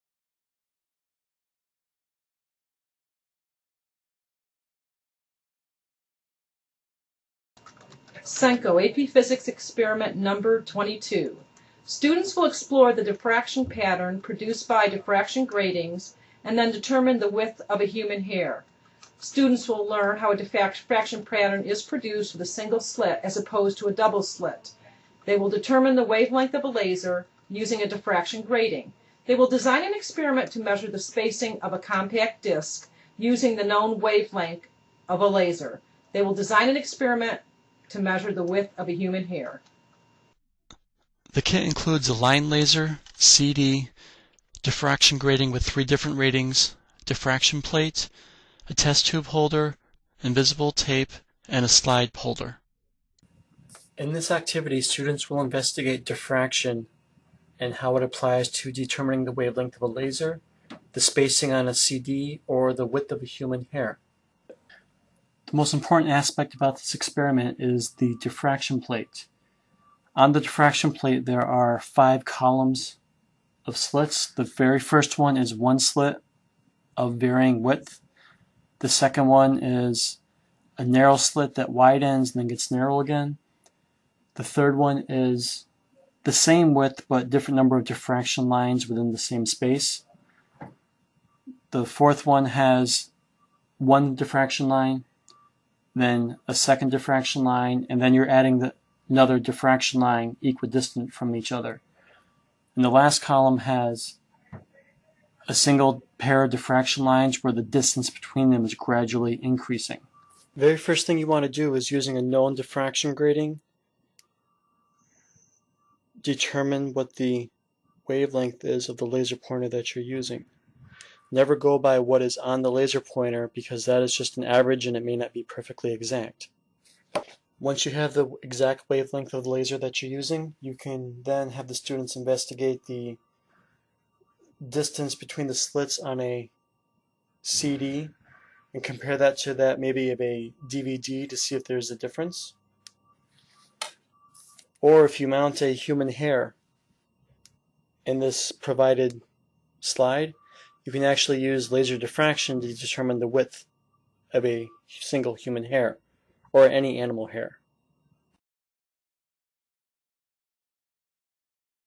AP Physics Lab 22: Diffraction
Students will explore the diffraction pattern produced by diffraction gratings and then determine the width of a human hair. By using a diffraction grating of known spacing, students will verify the wavelength of a laser pointer and use this to determine the spacing between the lines of a CD.

Ideal for for AP, B and C, IB, and advanced honors physics courses.